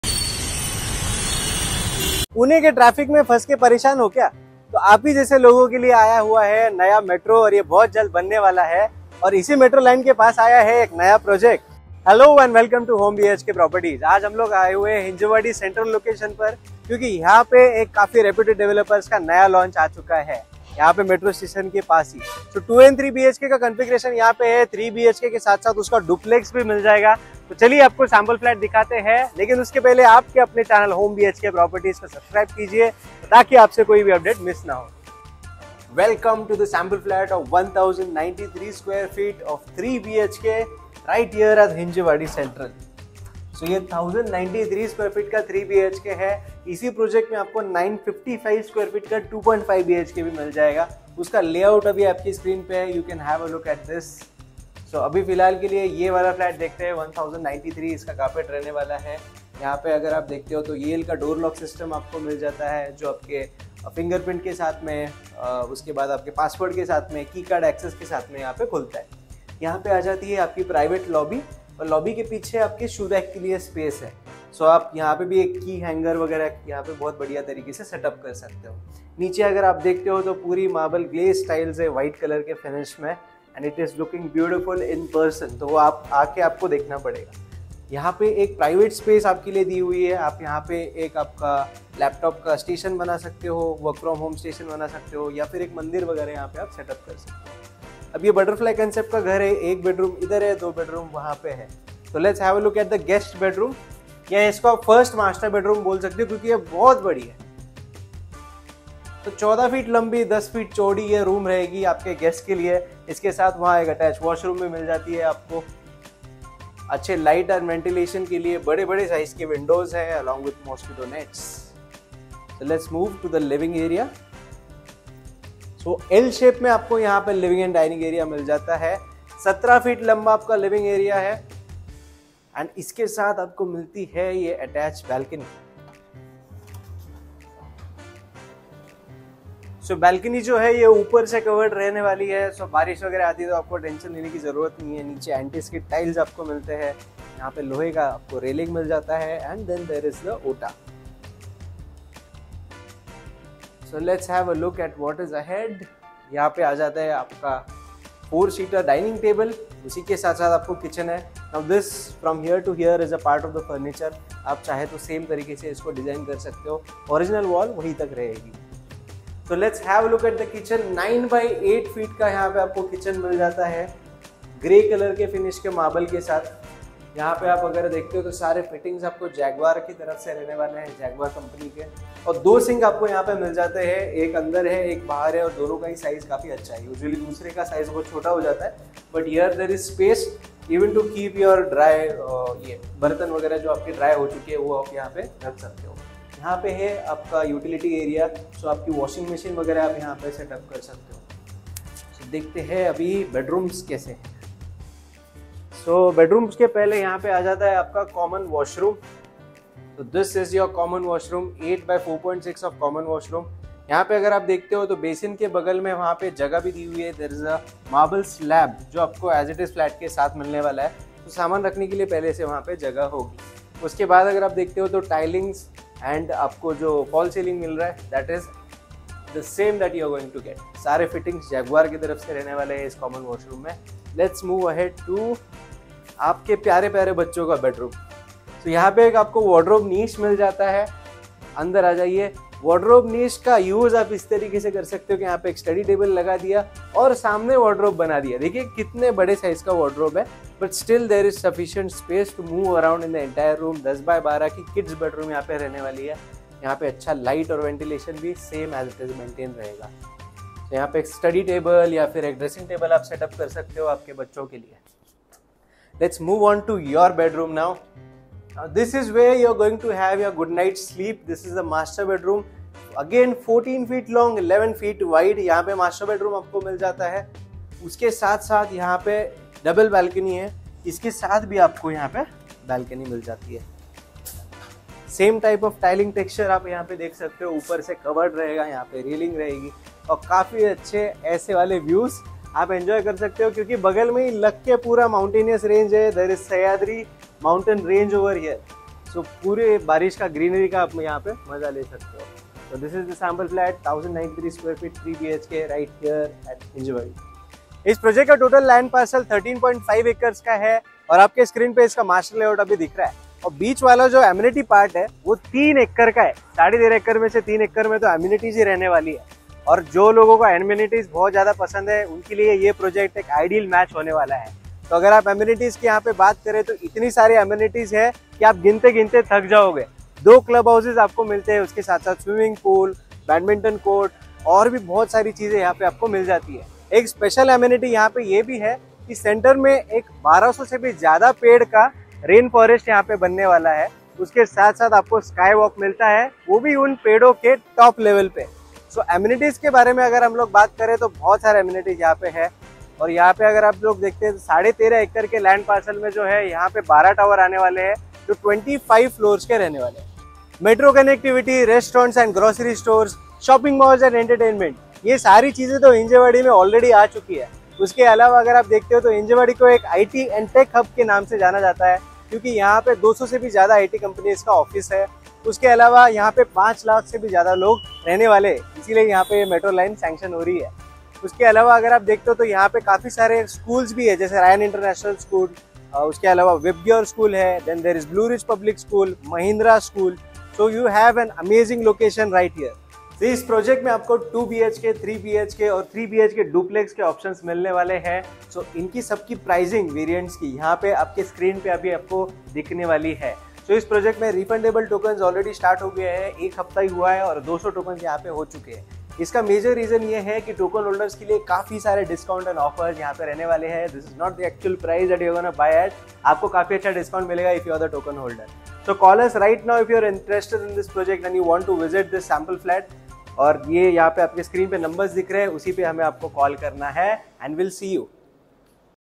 3 bhk in Hinjewadi Pune | Duplex Apartment | Largest Project of Hinjewadi | Project: H01
Welcome to the Best Project of Hinjewadi coming up with 12 Towers and Mind blowing Amenities!
No Brokerage is involved!
The configurations are:
2 BHK - 725 sq ft
2 BHK - 803 sq ft
2.5 BHK - 955 sq ft
3 BHK - 1087 sq ft
3 BHK Duplex - 1276 sq ft
Check Video For Prices

Amenities in 3.5 Acres:-
1. Swimming Pool
2. Rain Forest
3. Sky Walk
4. 2 Club House
5. Badminton Court
6. Basket Ball Court & 25 More!

Metro Station is at walkable distance. The locality is well developed and has all necessary requirements in the vicinity.